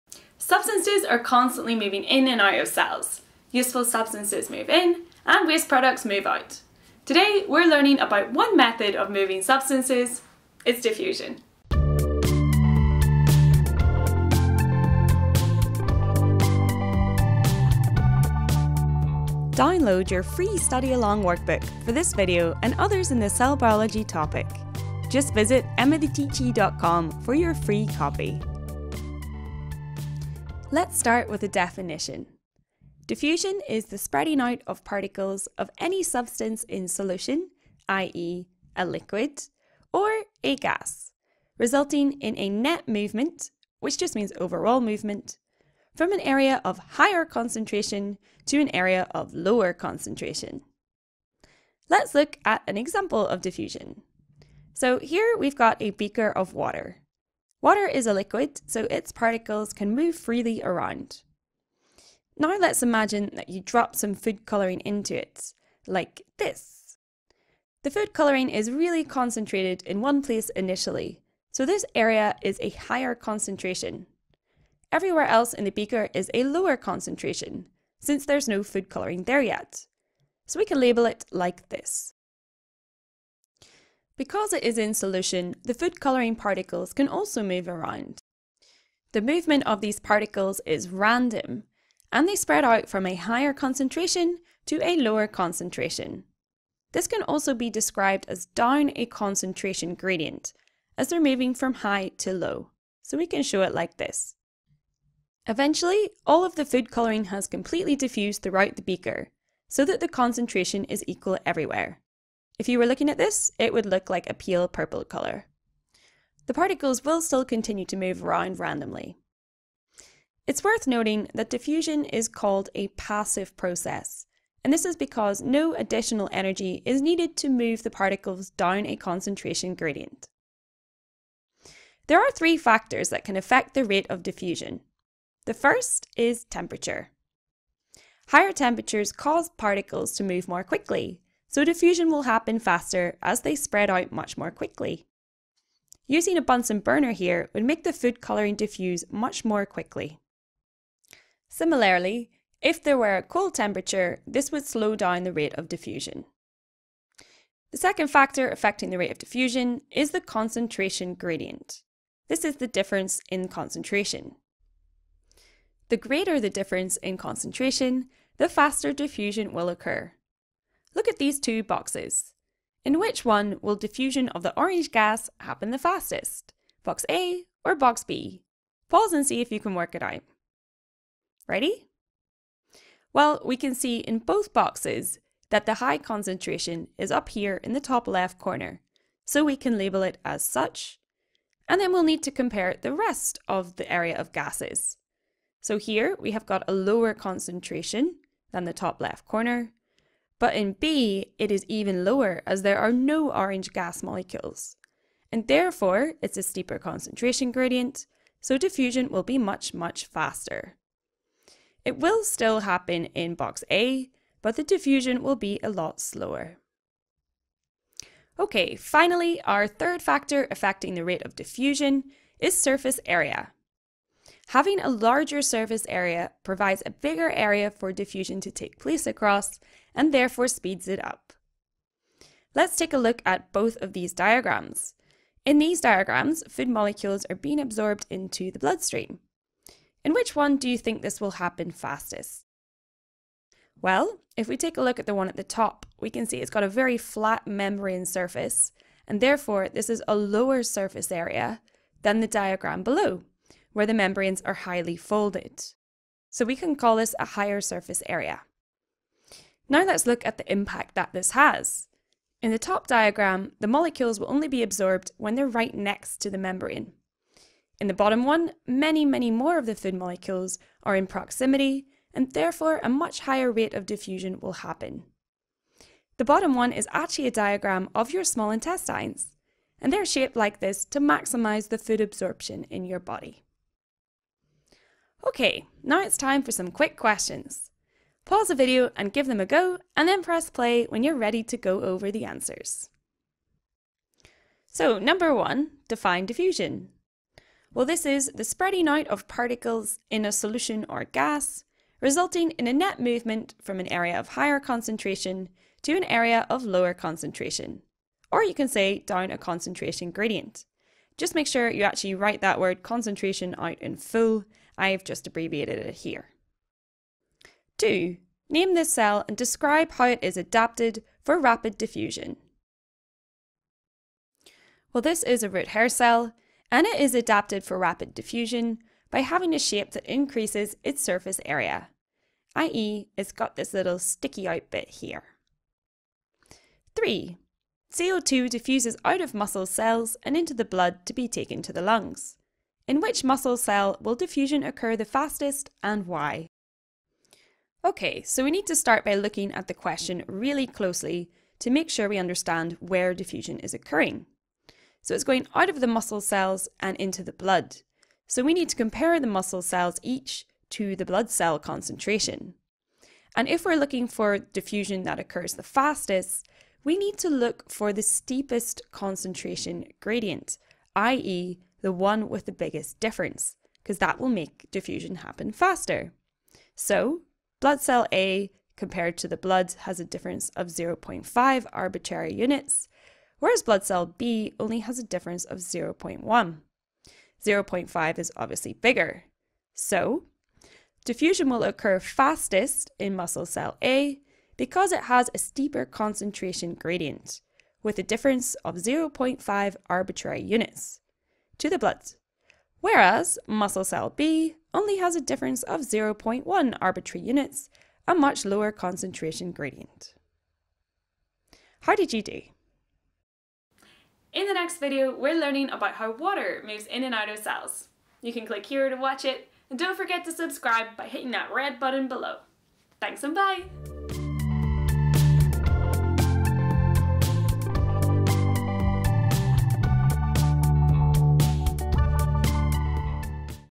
DIFFUSION GCSE Biology 9-1 | Combined Sci (Revision & Questions)
Get your free GCSE Biology Studyalong Workbook for this video - revision tasks, exam-style questions, progress tracker and more, it’ll boost your grades and confidence!

In this video, we’re looking at the process of diffusion - it’s the first of 3 transport methods in cells.

We’ll start with a definition, before looking at an example.

Then we’ll look at the 3 factors affecting the rate of diffusion and why they do this.

Finish up with some quick questions to test your understanding! Diffusion questions overlap a lot with osmosis and active transport, so make sure you revise these at the same time and can compare these processes.

I hope it helps you understand this topic a little better, let me know if you enjoyed it and what else you’d like to see from me!

Emma the teachie